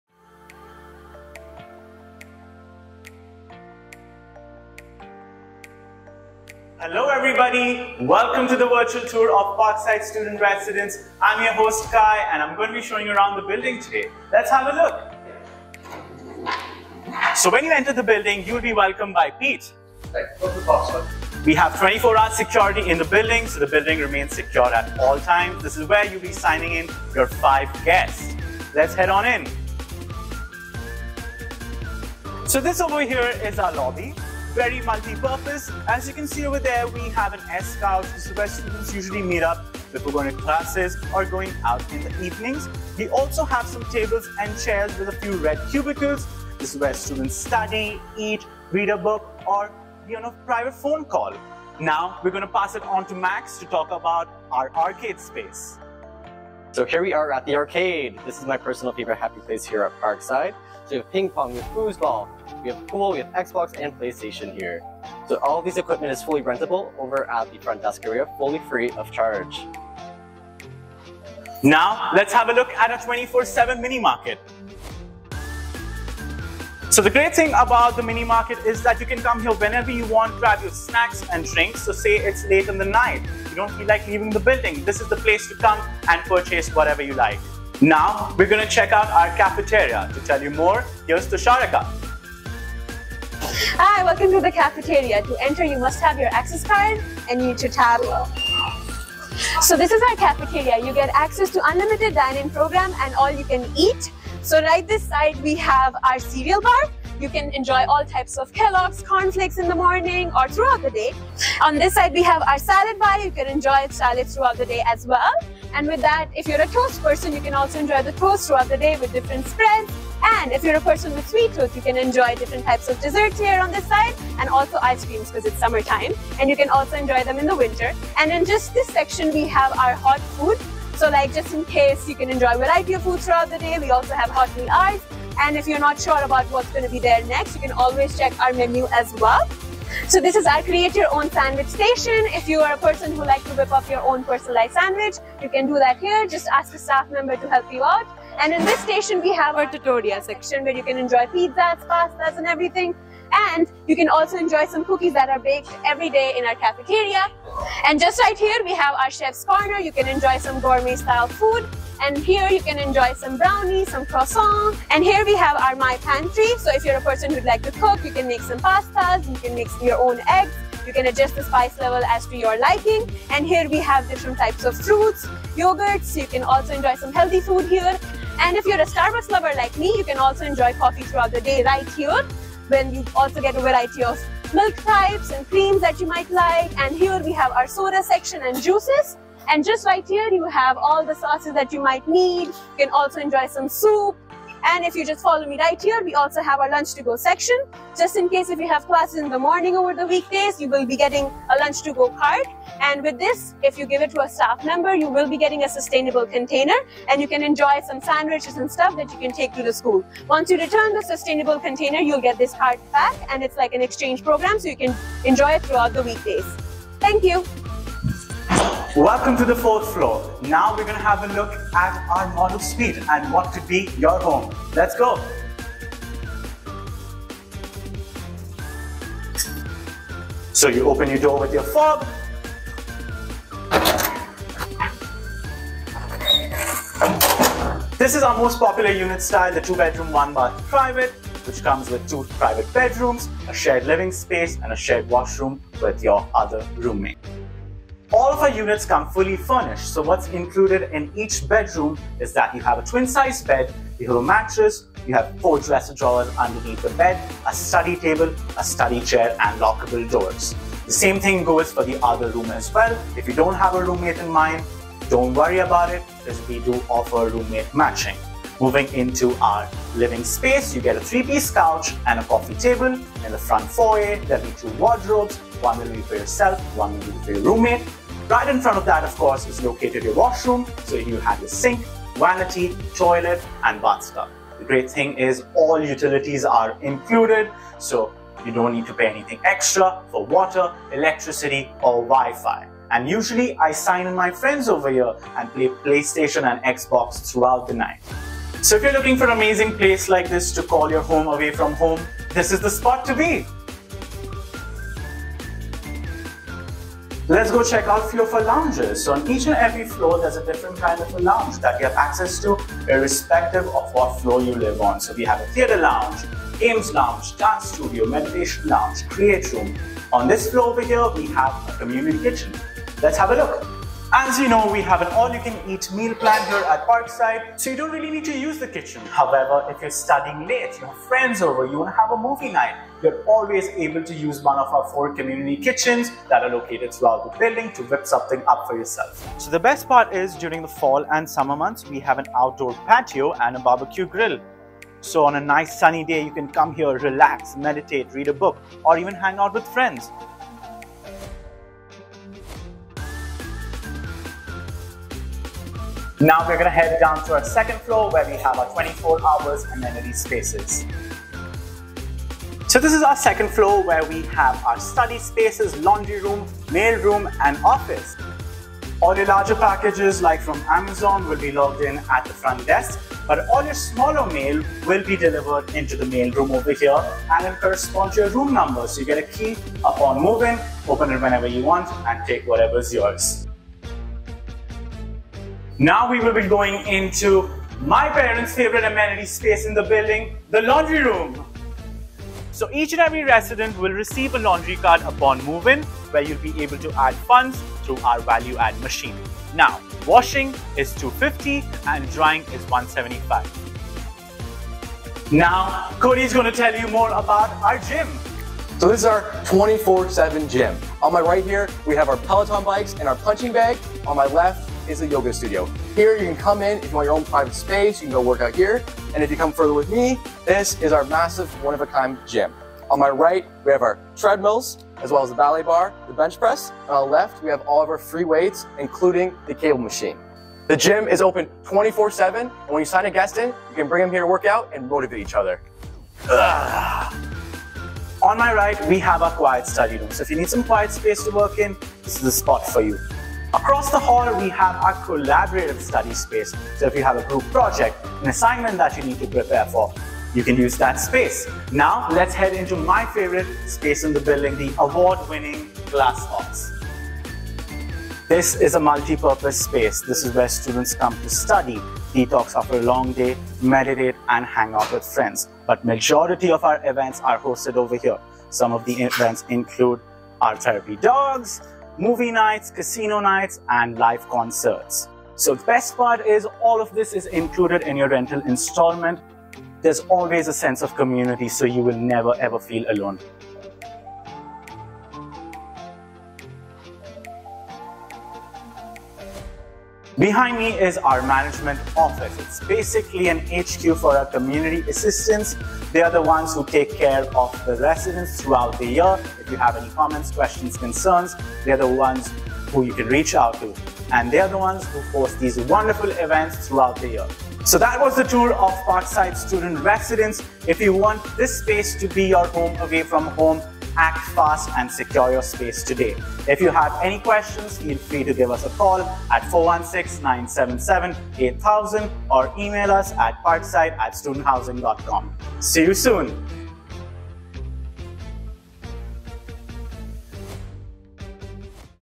Parkside Student Residence Tour 2023
Take a look inside Parkside Student residence in Downton Toronto.

Designed around domestic and international students of Toronto Metropolitan University (Formerly Ryerson University), U of T, OCAD, George Brown College and other nearby schools, Parkside is just steps from campus.

Enjoy state-of-the-art accommodations with a full dining program, 24-hour fitness centre featuring Peloton Bikes, dramatic study & lounge spaces, and an active social calendar – all in a perfect location in the heart of the city.